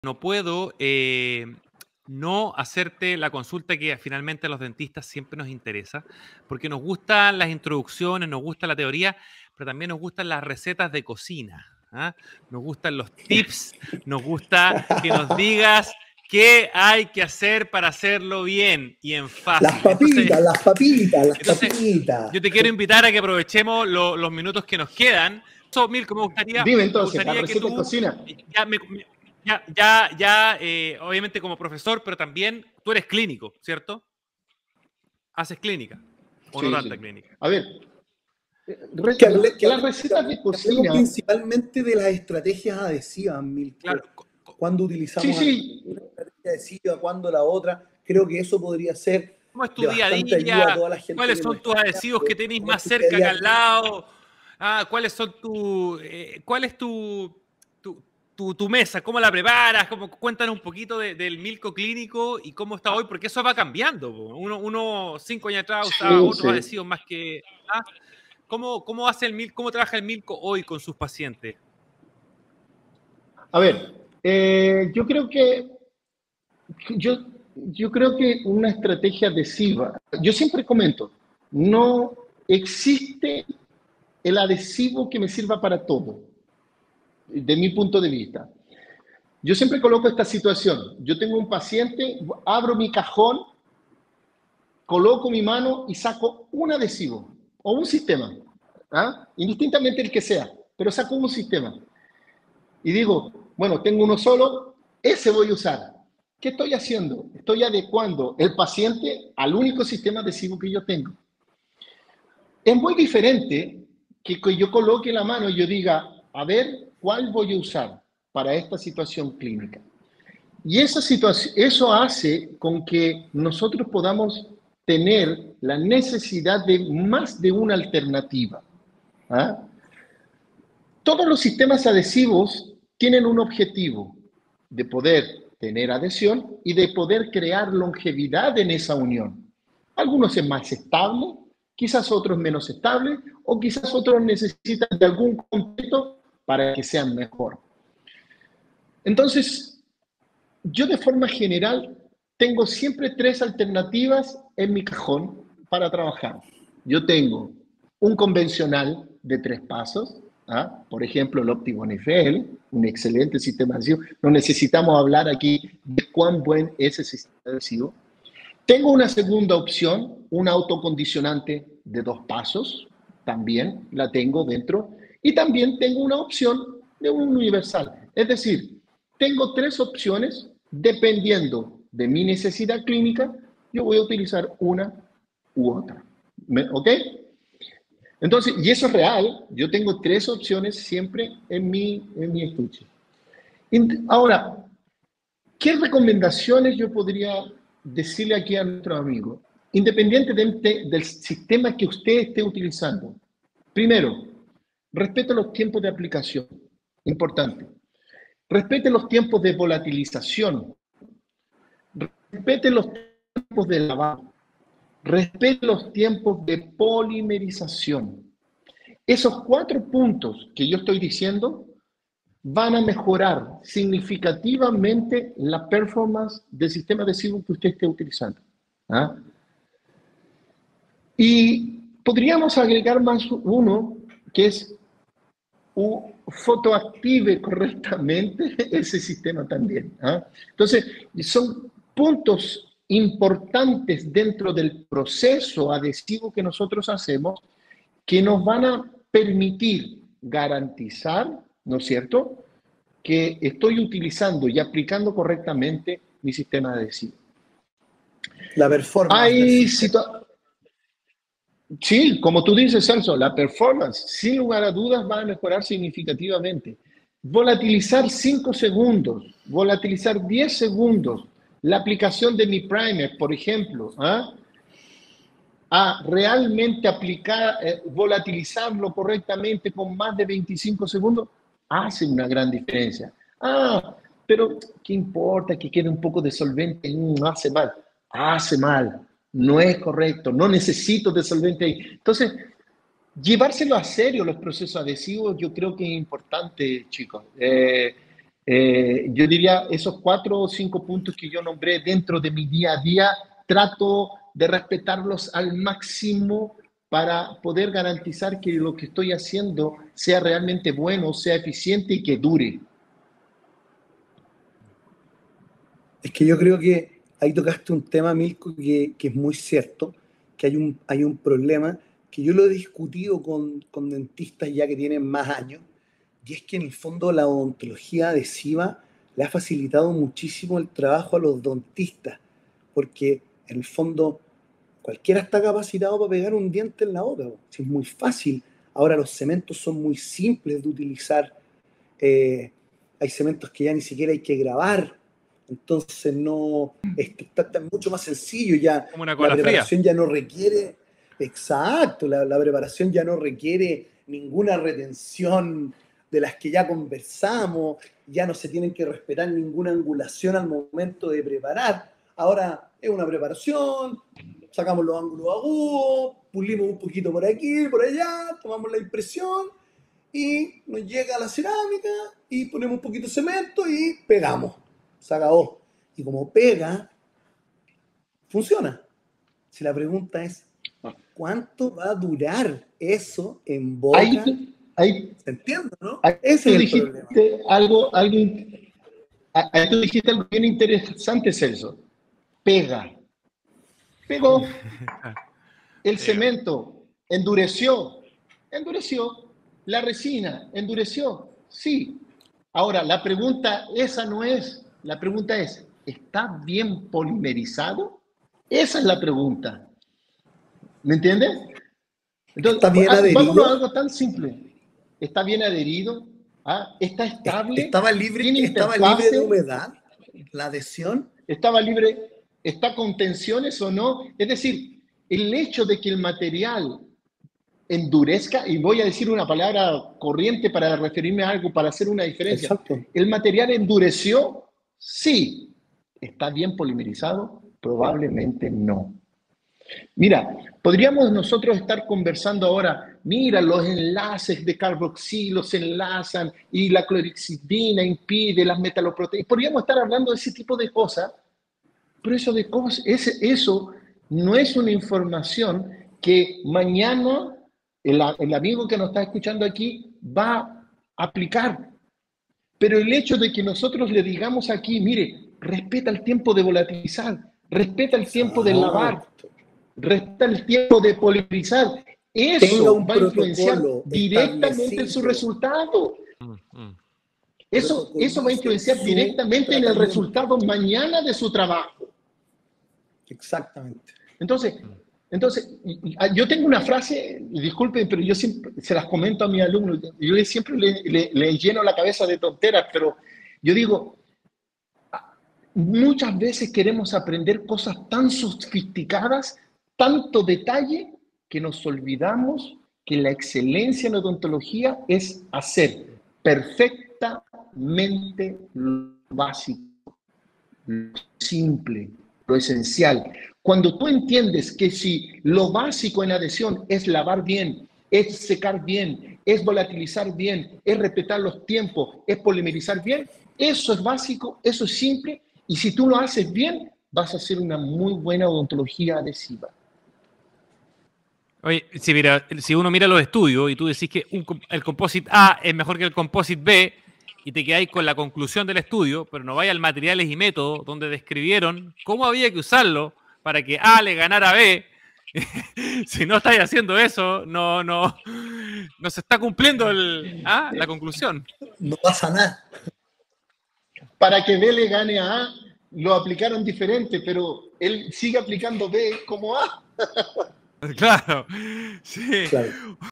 La receta adhesiva con el Dr. Milko Villarroel
Te invitamos a conocer la mejor receta adhesiva para nuestros procedimientos.
Acompáñanos junto al Dr Milko Villarroel 👨🏻‍⚕️​ y nutre tu técnica con las mejores recetas.

Coméntanos que te parece el material y si tienes alguna duda con gusto la responderemos.